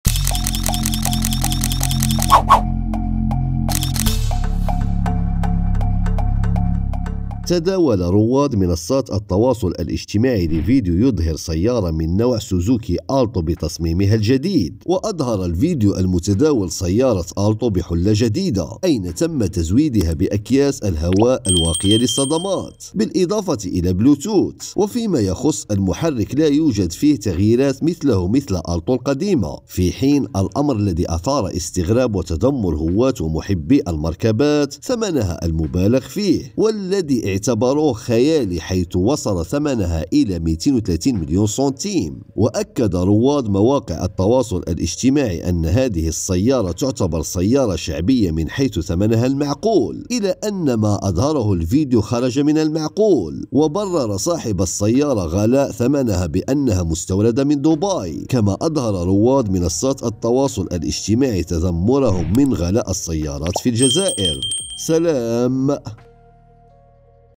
سعر سوزوكي ألتو الجديدة يصدم الجزائريين في رمضان
تداول رواد منصات التواصل الإجتماعي لفيديو يظهر سيارة من نوع سوزوكي ألتو ALTO  بتصميمها الجديد.

وأظهر الفيديو المتداول سيارة ألتو بحلة جديدة أين تم تزويدها بأكياس الهواء الواقية للصدمات. بالإضافة إلى البلوثوث. وفيما يخص المحرك لم يحدث المصنع فيه تغييرات مثله مثل ALTO القديمة.